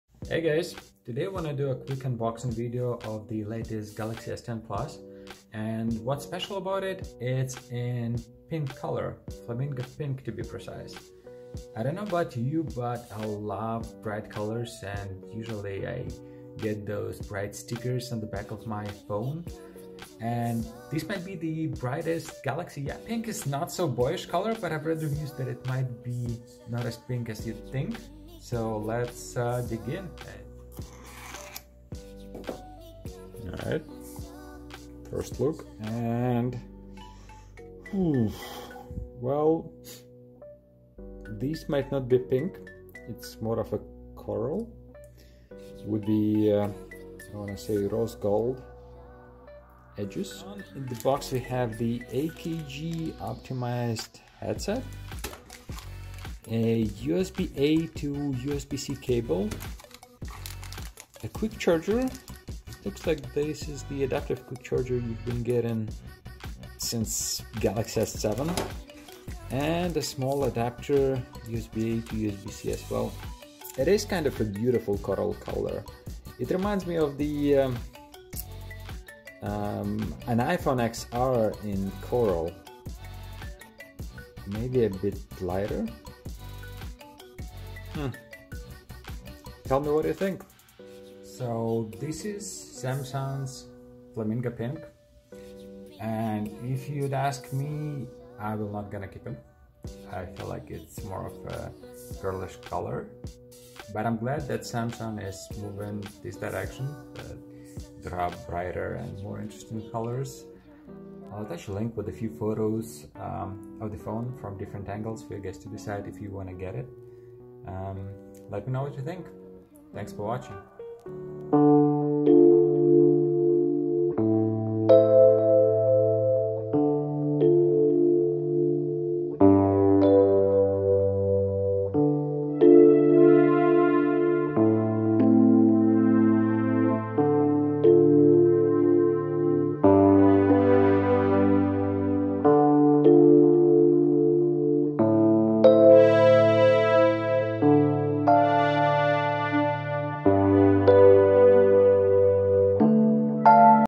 Unboxing of Galaxy S10+ in Flamingo Pink!
Here's a quick video to look at Samsung's latest flagship in a flamingo pink color.